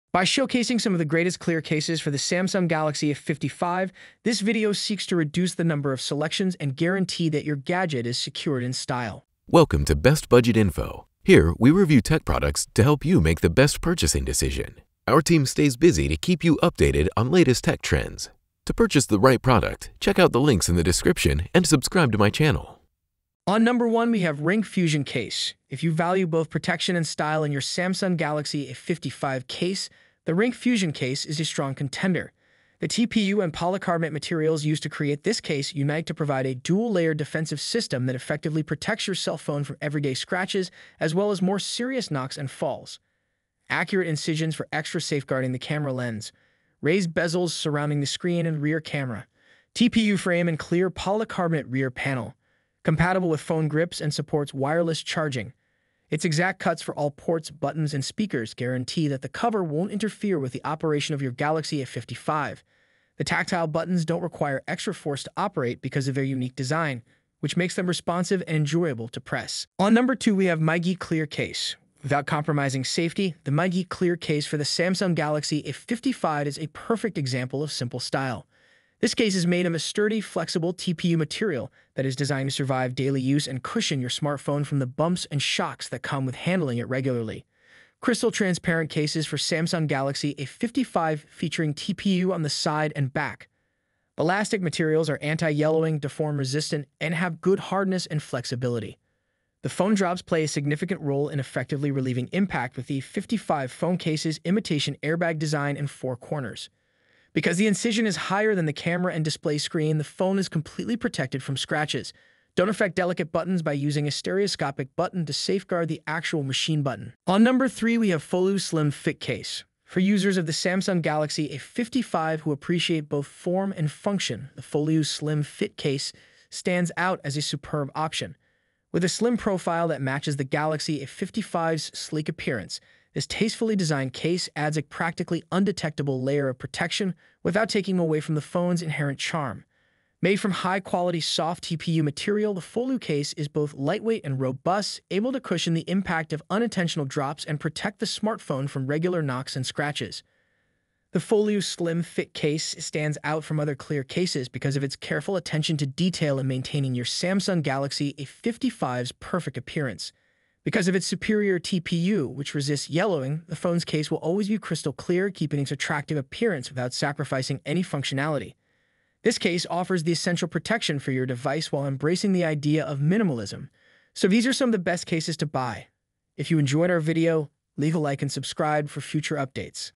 Best Clear cases for Samsung Galaxy A55 5G
By showcasing some of the greatest clear cases for the Samsung Galaxy A55, this video seeks to reduce the number of selections and guarantee that your gadget is secured in style.

More clear case

1. Ringke fusion clear case for Samsung A55

2. Migeec transparent case for Galaxy A55

3. Foluu slim fit clear case for Samsung A55